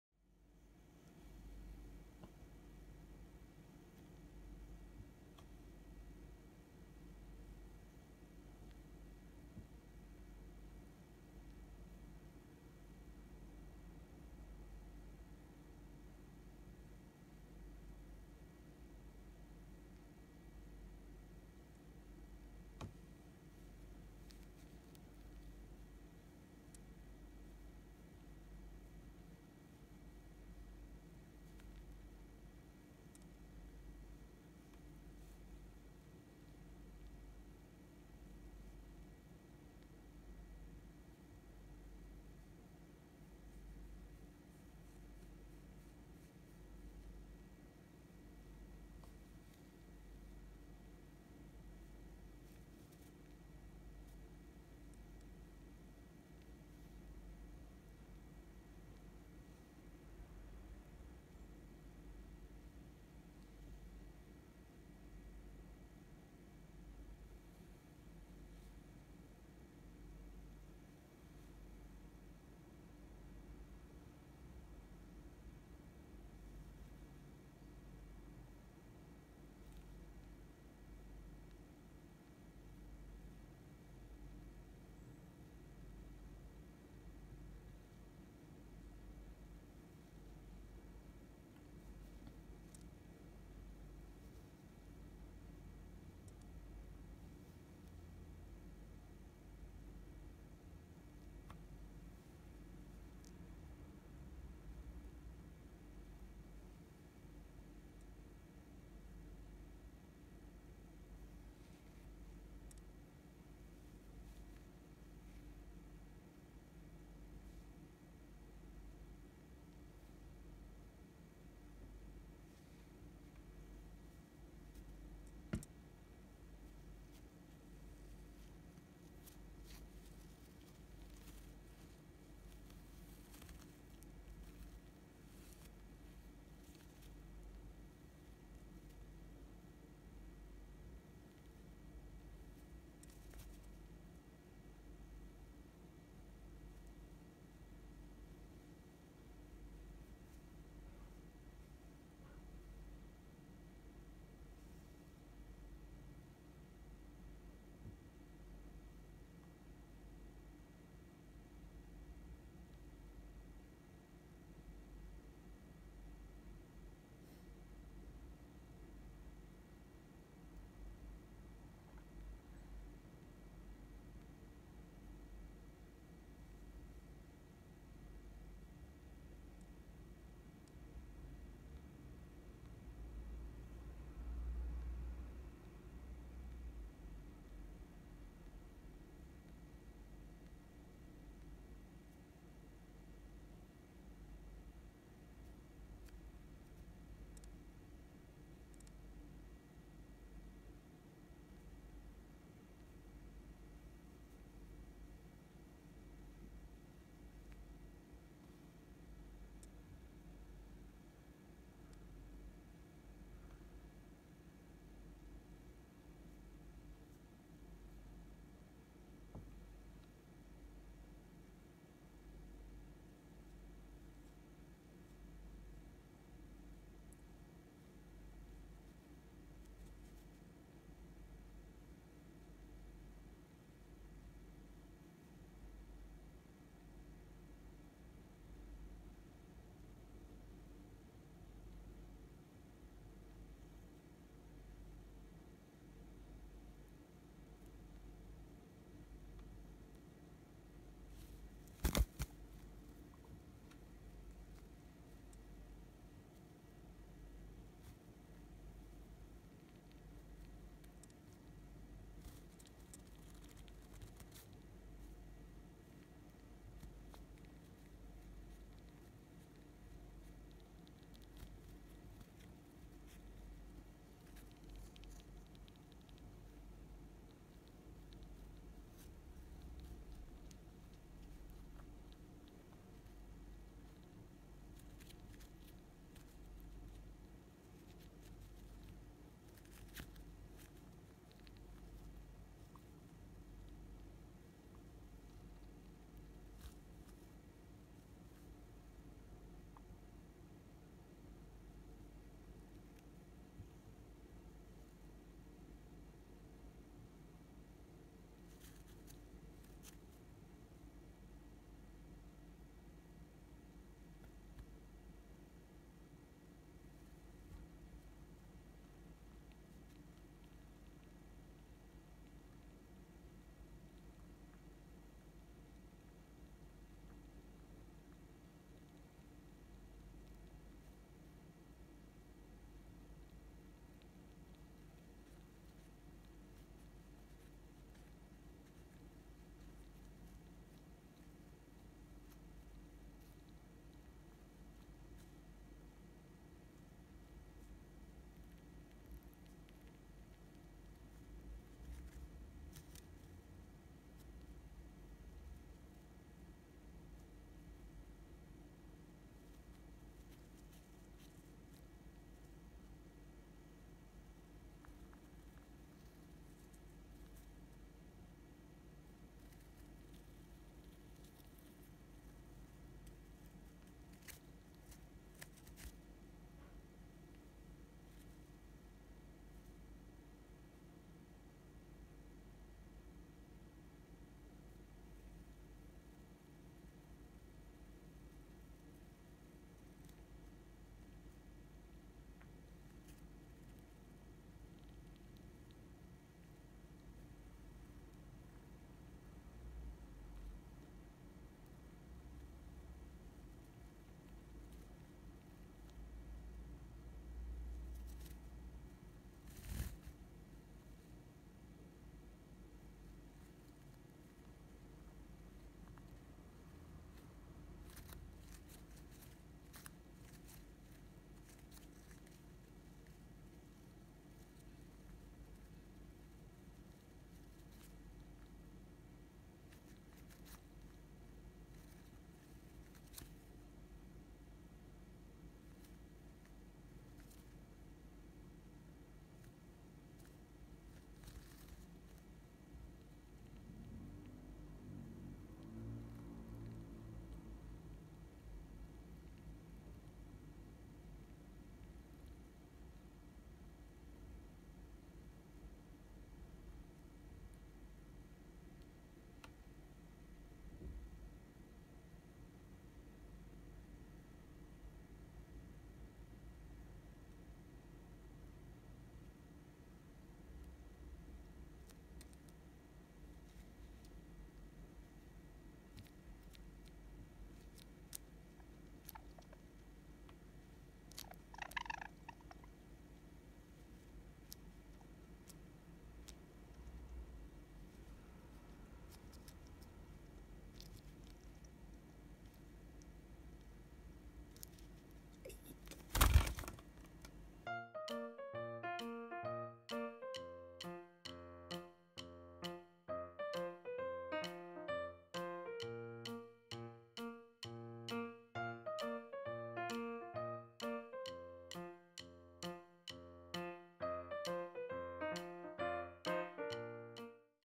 【文鳥】飼い主の愛情と信頼関係が伝わる動画【Java Finch】Video showing the love and trust of the owner
飼い主がひたすら文鳥を愛で続ける動画。羽の抜ける瞬間など。可愛すぎる♪

母の指にブチ切れ！飼い主の手を判断出来る文鳥のピョコたん【実家シリーズ】